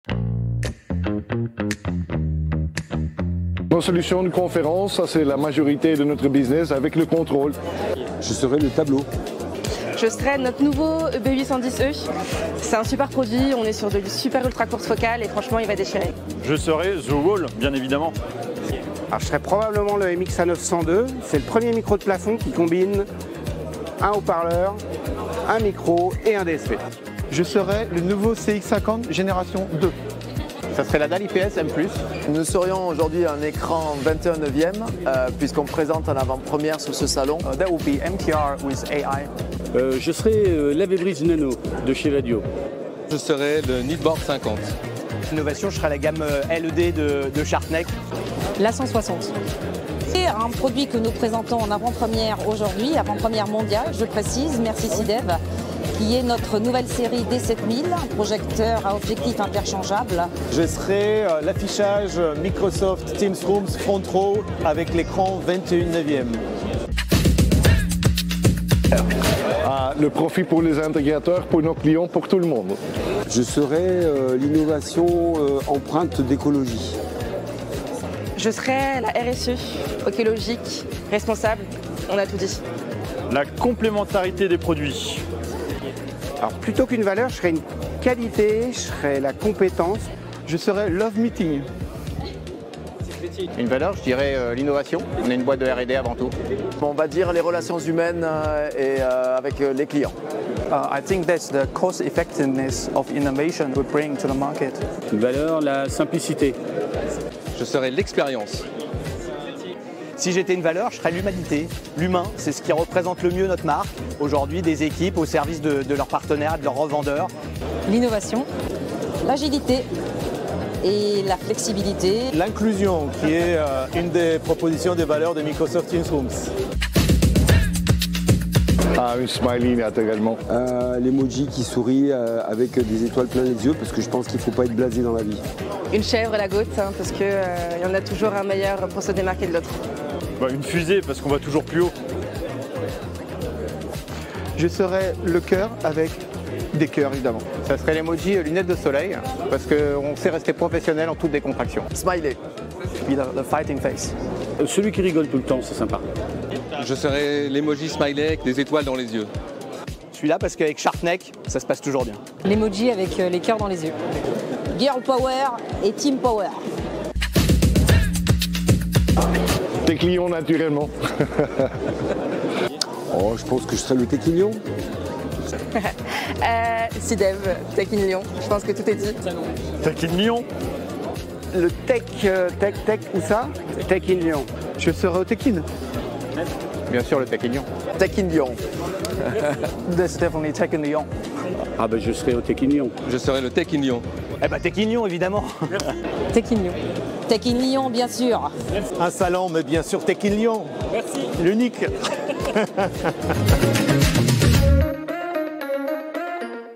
PORTRAIT CHINOIS - Interview Constructeurs Tech In Lyon 2023
TECHINLYON2023 - Interview constructeurs

Certains de nos partenaires se sont prêtés au jeu du Portrait Chinois lors de la 3ème édition de notre Salon TechinLyon, le 14 septembre dernier 🎥

Un exercice ludique, punchy et spontané qui nous permet de mieux les connaitre 😉 Merci à eux pour leur participation et leur bonne humeur Joris Cornelissens, Philippe BARACETTI, Soninka Yemy, Eric Erbrech, Guillaume Le Royer, Christophe Marchewka, Frédéric PINQUET, Frédéric Serafin, Patrick Li, Cyrille Bégorre, Jean-Christophe Bouillot, Gary Guillier-Marcellin, Audrey Allaire, Karen Mettoudi, Christophe Cygan

Découvrez-vite la breaking news à la fin de la vidéo 🎉

Et vous ? Si vous étiez un émoji vous seriez ? 😍